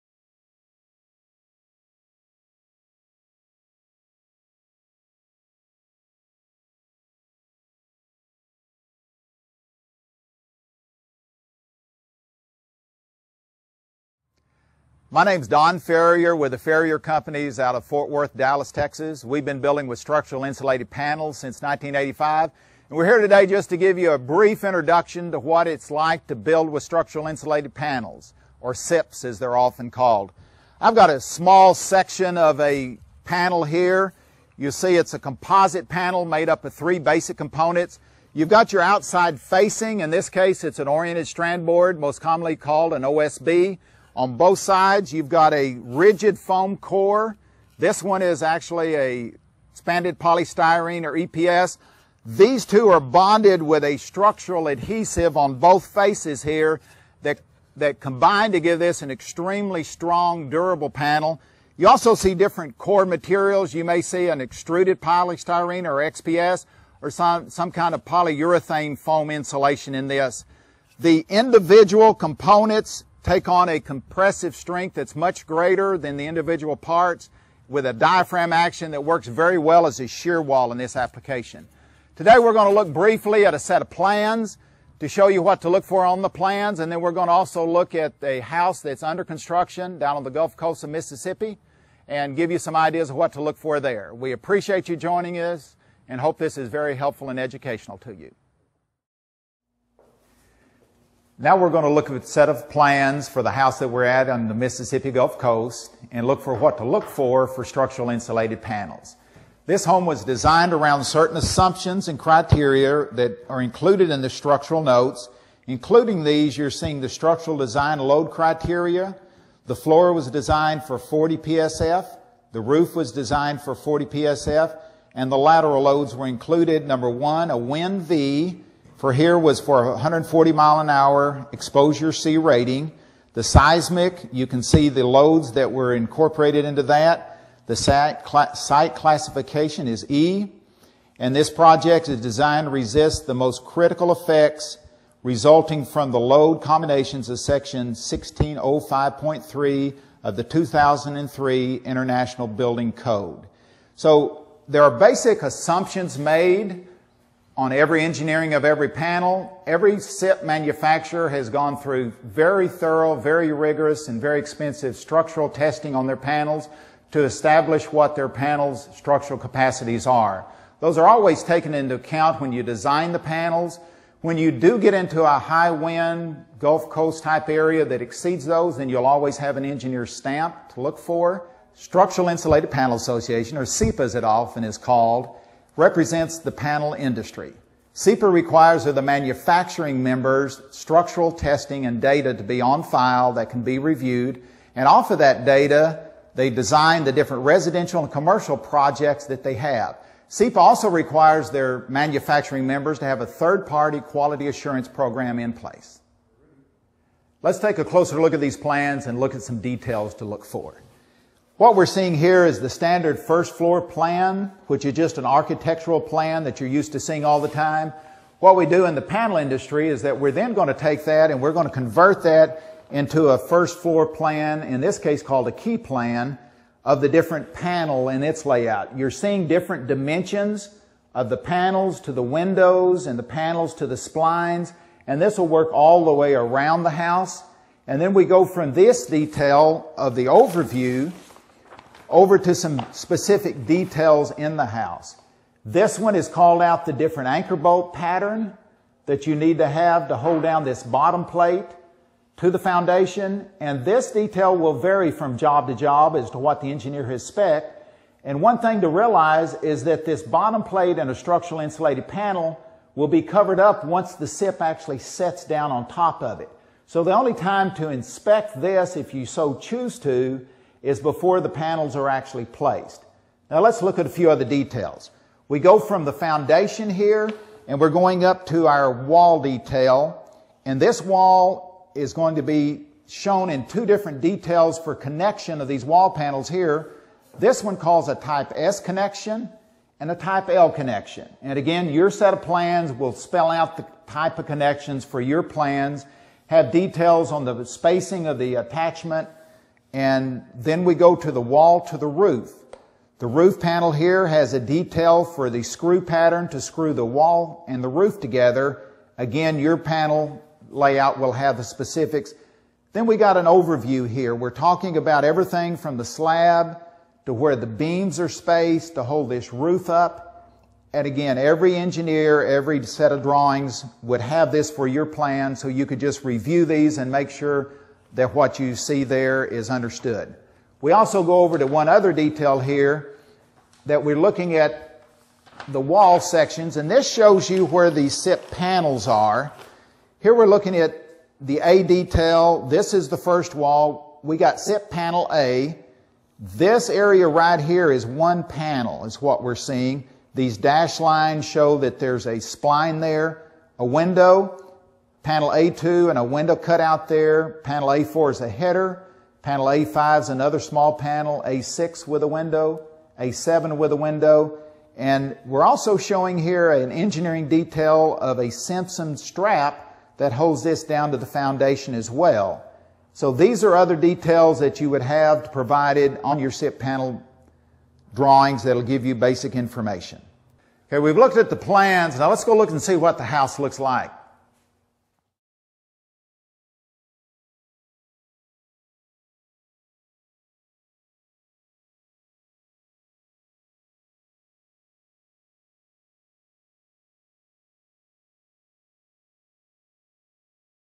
Looking at a SIP House-inspecting the plans
A review of OSB-faced structural insulated panel (SIP) and the building plans for a SIP system house in the Gulf Coast by Don Ferrier for the Structural Insulated Panels Association (SIPA).